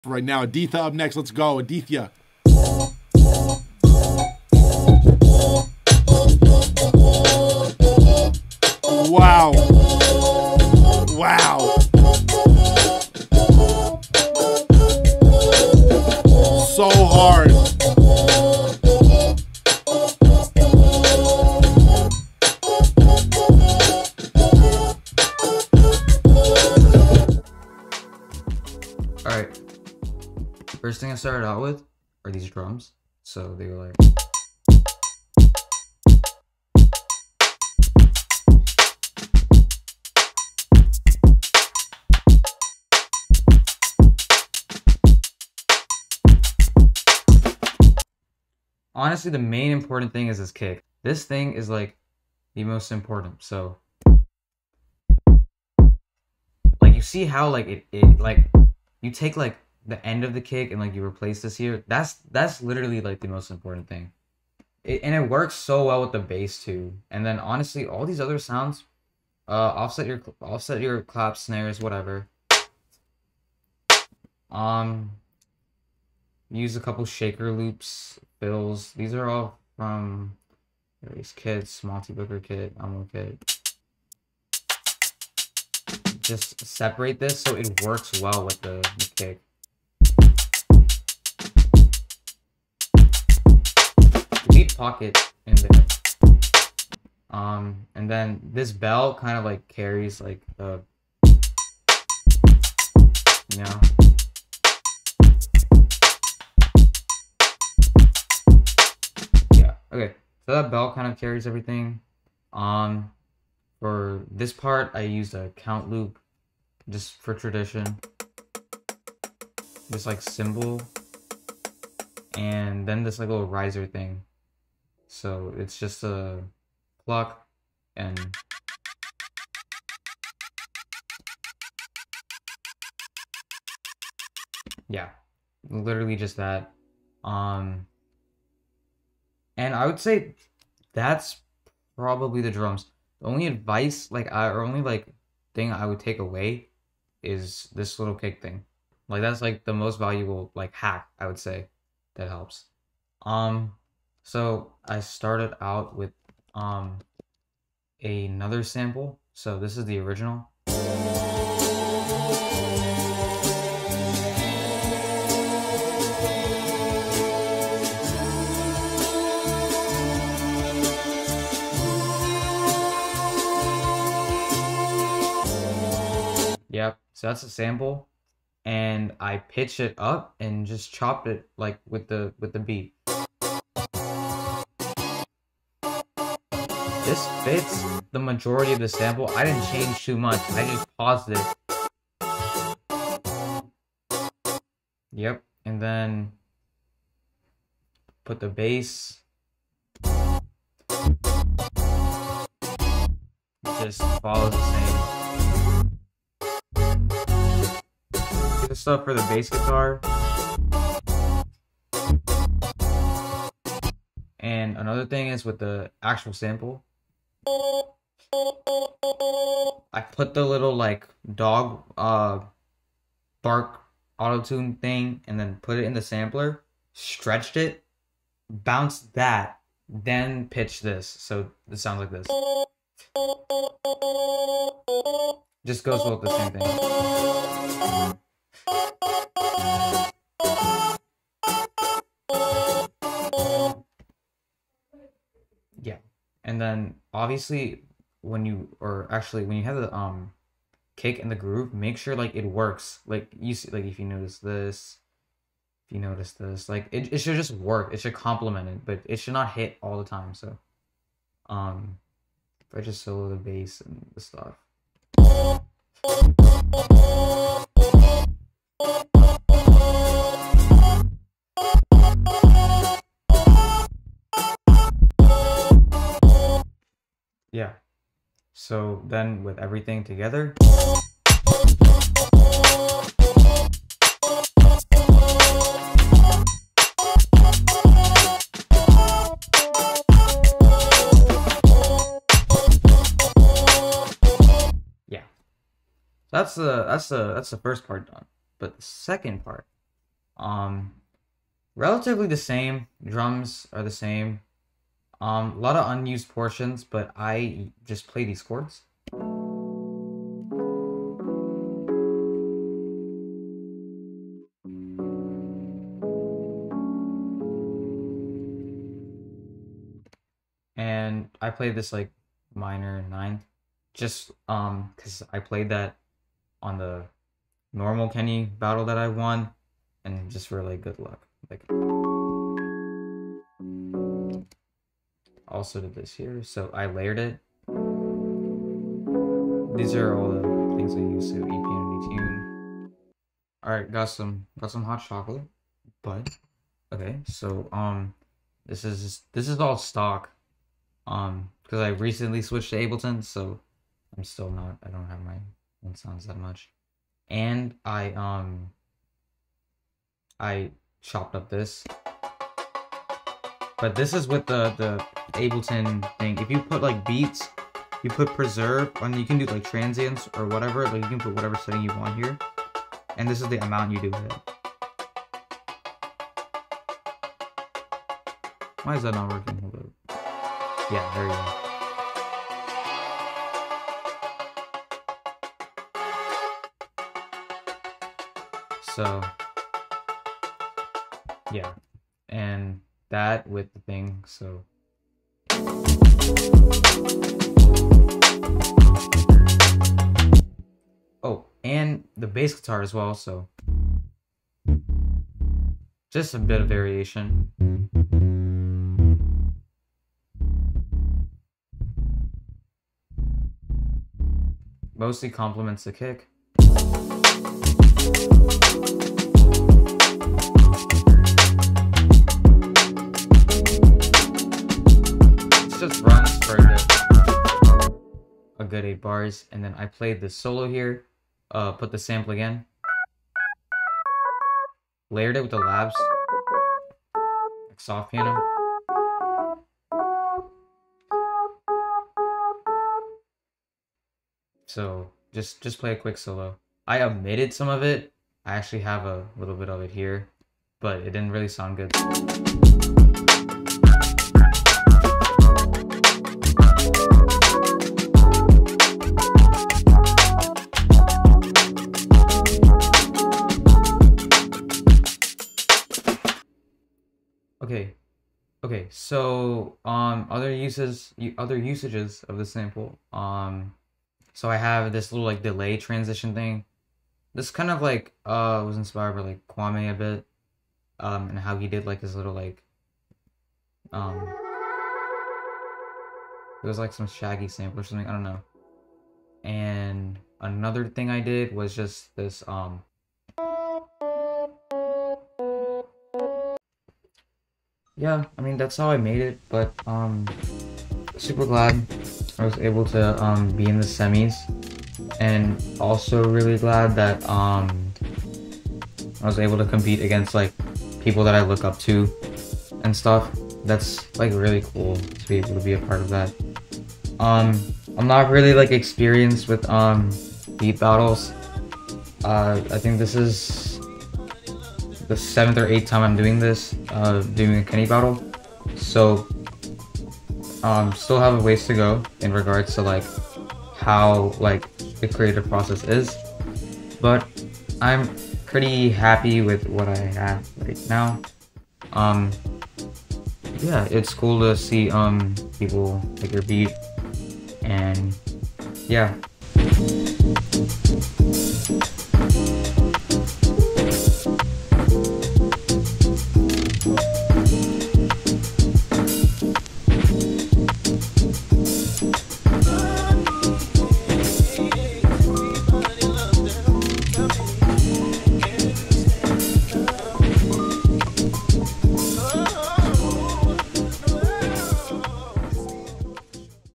how a 16 year old won the kenny beats semifinals (+ got into finals)
im really bad at editing and stuff so sorry if this isnt the best quality

yes ik i put my age there for views haha

also idk why this rendered in 360p sorry